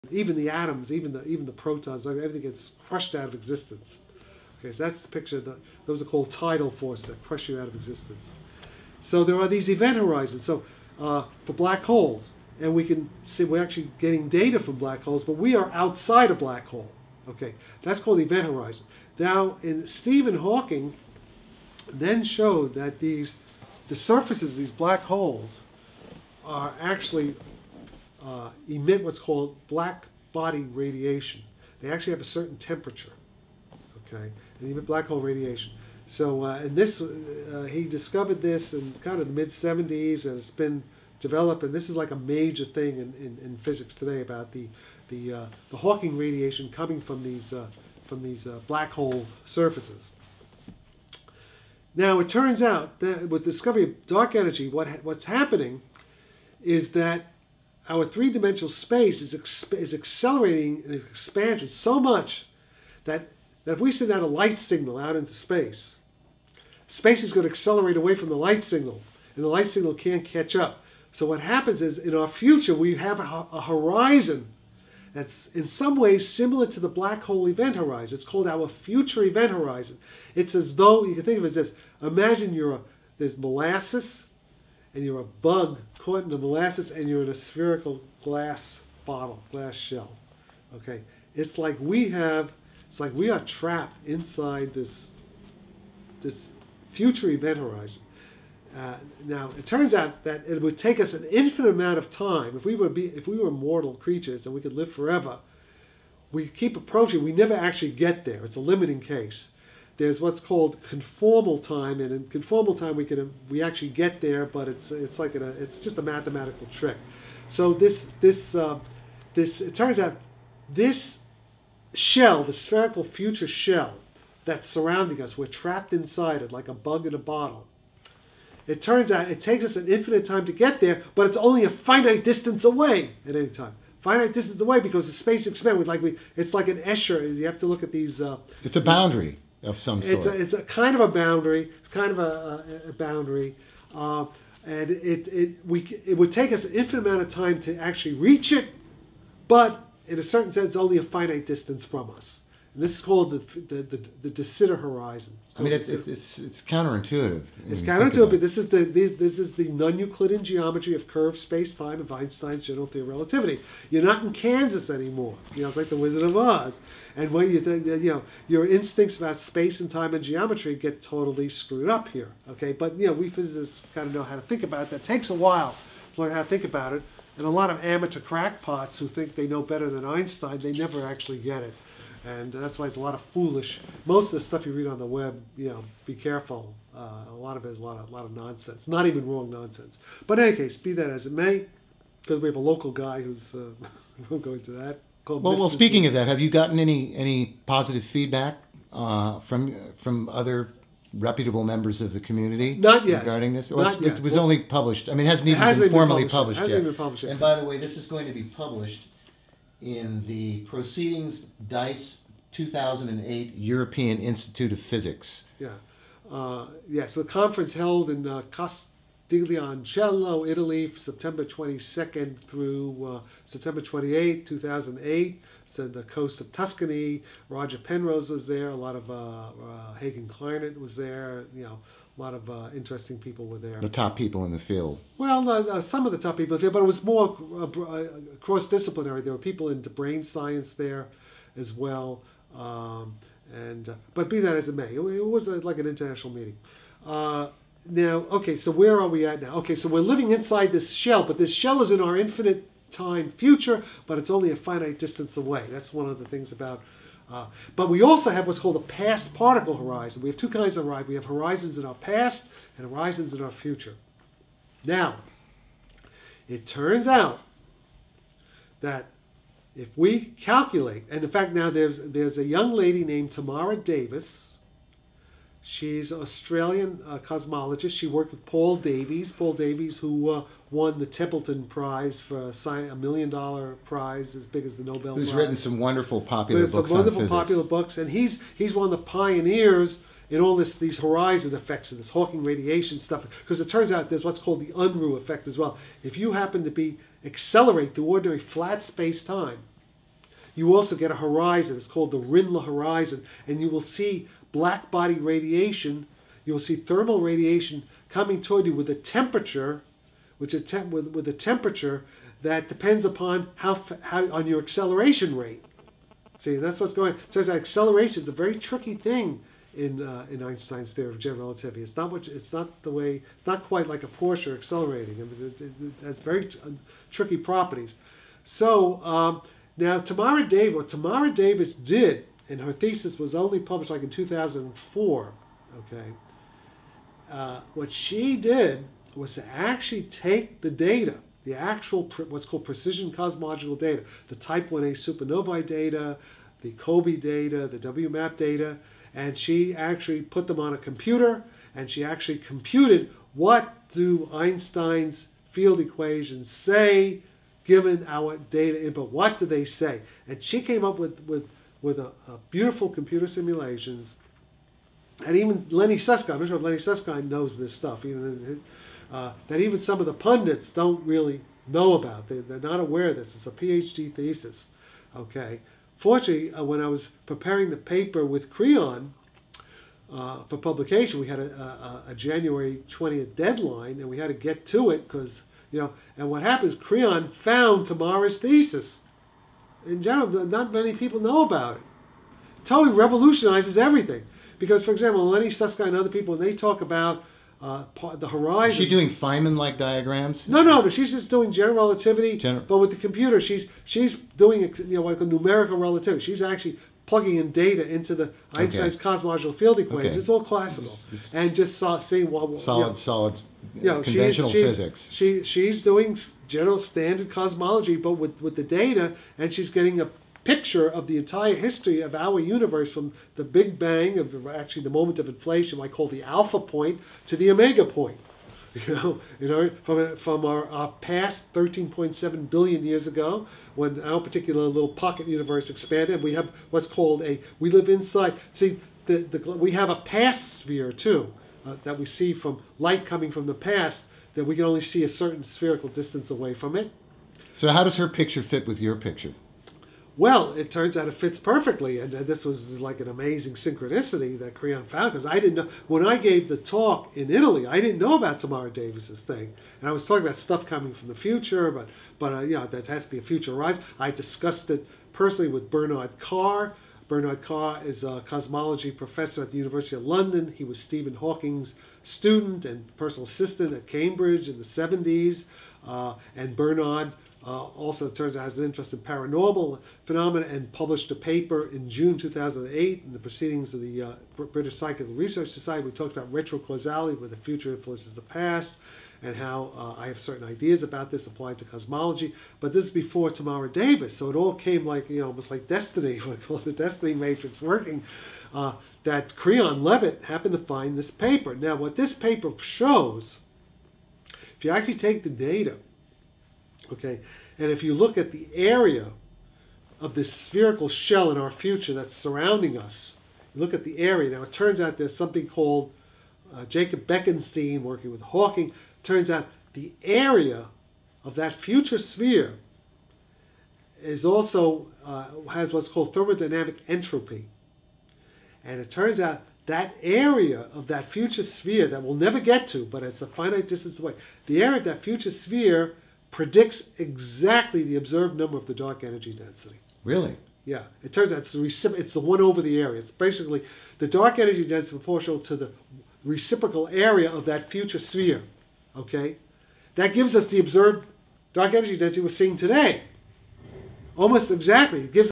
Further Conversations With Dr. Jack Sarfatti -- 3/18/09 -- Pt4
We discuss the theoretical and physical implications of Dr. Jack Sarfatti's latest research into the origins and nature of gravity, dark matter and dark energy.  Dr. Sarfatti's research has potentially great significance for new forms of energy production and exotic propulsion, applicable to star travel.